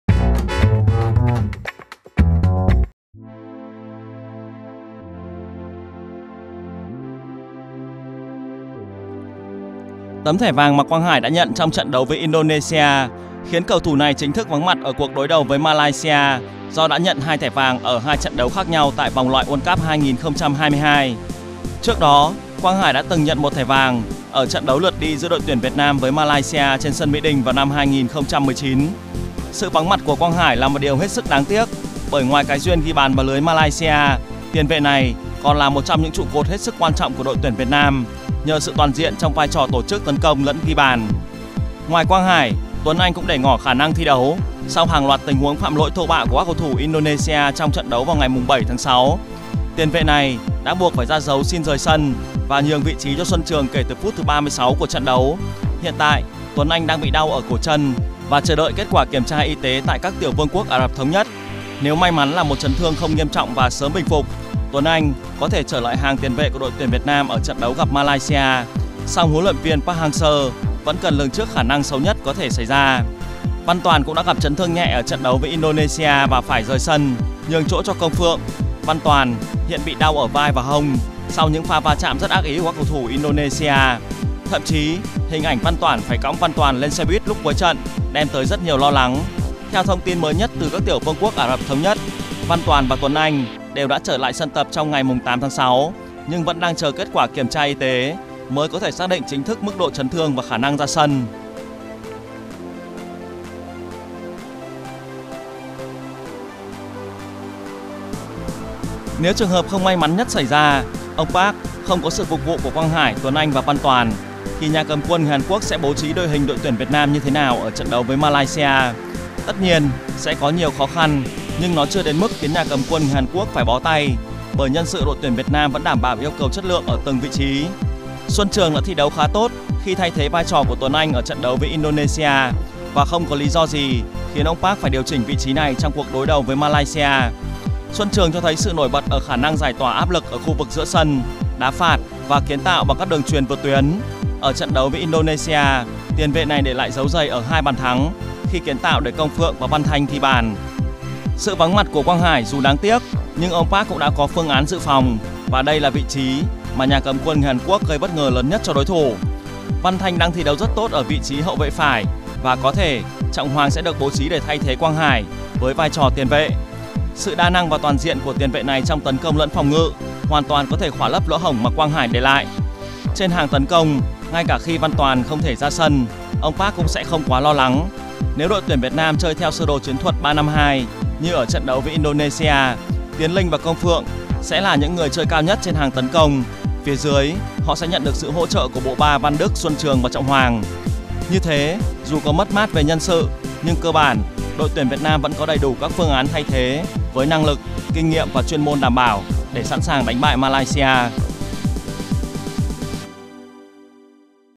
Đội tuyển Việt Nam mất 3 trụ cột ở trận gặp Malaysia, thầy Park đau đầu? VÒNG LOẠI WORLD CUP 2022
Ngoài Quang Hải chắc chắn vắng mặt do nhận 2 thẻ vàng, chấn thương của Tuấn Anh và Văn Toàn khiến 2 cầu thủ này để ngỏ khả năng thi đấu. Trong trường hợp xấu nhất xảy ra, ông Park sẽ giải bài toán nhân sựcủa đội tuyển Việt Nam như thế nào ở trận đấu gặp Malaysia?
* Đội tuyển Việt Nam mất 3 trụ cột
Tấm thẻ vàng mà Quang Hải đã nhận trong trận đấu với Indonesia khiến cầu thủ này chính thức vắng mặt ở cuộc đối đầu với Malaysia do đã nhận 2 thẻ vàng ở trận đấu khác nhau tại vòng loại World Cup 2022. Trước đó, Quang Hải đã từng nhận 1 thẻ vàng ở trận đấu lượt đi  giữa đội tuyển Việt Nam với Malaysia trên sân Mỹ Đình vào năm 2019.
Sự vắng mặt của Quang Hải là một điều hết sức đáng tiếc, bởi ngoài cái duyên ghi bàn vào lưới Malaysia, tiền vệ này là một trong những trụ cột hết sức quan trọng của đội tuyển Việt Nam nhờ sự toàn diện trong vai trò tổ chức tấn công lẫn ghi bàn.
Ngoài Quang Hải, Tuấn Anh cũng để ngỏ khả năng thi đấu sau hàng loạt tình huống phạm lỗi thô bạo của các cầu thủ Indonesia trong trận đấu vào ngày 7/6. Tiền vệ này đã buộc phải ra dấu xin rời sân và nhường vị trí cho Xuân Trường kể từ phút thứ 36 của trận đấu.
Văn Toàn cũng đã gặp chấn thương nhẹ ở trận đấu với Indonesia và phải ra sân, nhường chỗ cho Công Phượng. Văn Toàn hiện bị đau ở vai và hông sau nhưng pha va chạm rất ác ý của các cầu thủ Indonesia.
Theo thông tin mới nhất từ UAE, Văn Toàn và Tuấn Anh đều đã trở lại sân tập trong ngày 8/6 nhưng vẫn đang chờ kết quả kiểm tra y tế, mới có thể xác định chính xác mức độ chấn thương và khả năng ra sân.
* Ông Park sẽ gây bất ngờ cho Malaysia
Nếu trường hợp không may mắn nhất xảy ra, ông Park không có sự phục vụ của Quang Hải, Tuấn Anh và Văn Toàn, thì nhà cầm quân người Hàn Quốc sẽ bố trí đội hình đội tuyển Việt Nam như thế nào ở trận đấu với Malaysia? Tất nhiên, sẽ có nhiều khó khăn nhưng nó chưa đến mức khiến nhà cầm quân người Hàn Quốc phải bó tay, bởi nhân sự đội tuyển Việt Nam vẫn đảm bảo yêu cầu về chất lượng nhân sự ở từng vị trí.
Xuân Trường đã thi đấu khá tốt khi thay thế vai trò của Tuấn Anh ở trận đấu với Indonesia và không có lý do gì khiến ông Park phải điều chỉnh vị trí này trong cuộc đối đầu với Malaysia. Xuân Trường cho thấy sự nổi bật ở khả năng giải tỏa áp lực ở khu vực ở giữa sân, đá phạt và kiến tạo bằng các đường chuyền vượt tuyến. Ở trận đấu với Indonesia, tiền vệ này để lại dấu giày ở 2 bàn thắng khi kiến tạo để Công Phượng và Văn Thanh ghi bàn.
Sự vắng mặt của Quang Hải dù đáng tiếc nhưng ông Park cũng đã có phương án dự phòng và đây là vị trí nhà cầm quân người Hàn Quốc gây bất ngờ lớn nhất cho đối thủ. Văn Thanh đang thi đấu rất tốt ở vị trí hậu vệ phải và có thể Trọng Hoàng sẽ được bố trí thay thế Quang Hải với vai trò tiền vệ. Sự đa năng và toàn diện của tiền vệ này trong tấn công lẫn phòng ngự, hoàn toàn có thể khỏa lấp lỗ hổng mà Quang Hải để lại.
Trên hàng tấn công, ngay cả khi Văn Toàn không thể ra sân, ông Park cũng sẽ không quá lo lắng. Nếu đội tuyển Việt Nam chơi theo sơ đồ chiến thuật 3-5-2 như ở trận đấu với Indonesia, Tiến Linh và Công Phương sẽ là những người chơi cao nhất trên hàng tấn công. Phía dưới, họ sẽ nhận được sự hỗ trợ của bộ ba Văn Đức - Xuân Trường - Trọng Hoàng.
Như thế, dù có mất mát về nhân sự nhưng cơ bản, đội tuyển Việt Nam vẫn có đầy đủ các phương án thay thế với năng lực, kinh nghiệm và chuyên môn đảm bảo để sẵn sàng đánh bại Malaysia.
* Lịch thi đấu, kết quả, lịch truyền hình trực tiếp vòng loại World Cup 2022 khu vực châu Á bảng G:
- 23h45 ngày 3/6: UAE vs Malaysia 4-0
- 23h45 ngày 3/6: Thái Lan vs Indonesia 2-2
- 23h45 ngày 7/6 Việt Nam vs Indonesia 4-0
- 23h45 ngày 7/6 UAE vs Thái Lan 3-1
- 23h45 ngày 11/6 Malaysia vs Việt Nam (trực tiếp VTV6, VTV5)
- 23h45 ngày 11/6 Indonesia vs UAE
- 23h45 ngày 15/6 UAE vs Việt Nam (trực tiếp VTV6, VTV5)
- 23h45 ngày 15/6 Thái Lan vs Malaysia